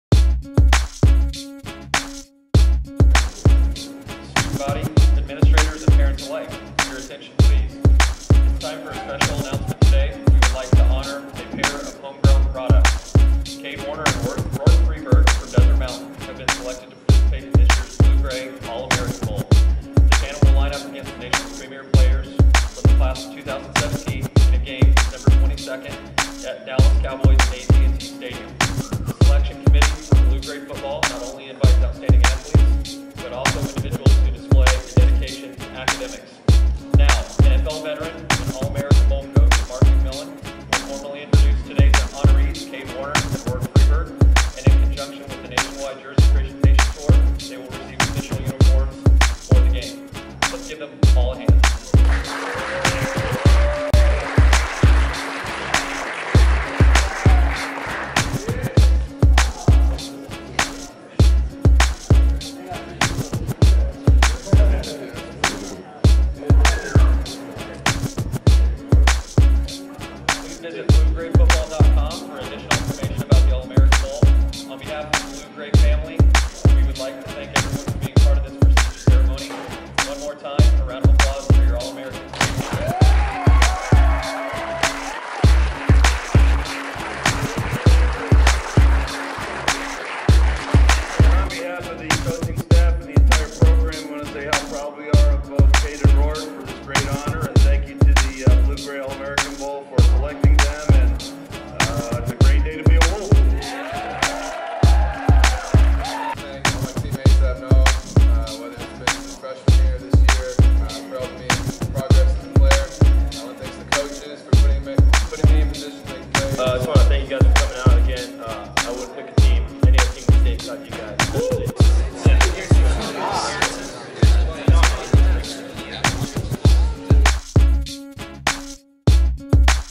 Blue-Grey Presentation Tour: Kade Warner and Rourke Freeburg
During the recent nationwide Blue-Grey Jersey Presentation Tour, in advance of the brand's All-American Bowl, Kade Warner and Rourke Freeburg from Desert Mountain (AZ) were recognized in ceremony with NFL veteran Mark McMillian. Sean Ceglinsky produced the video.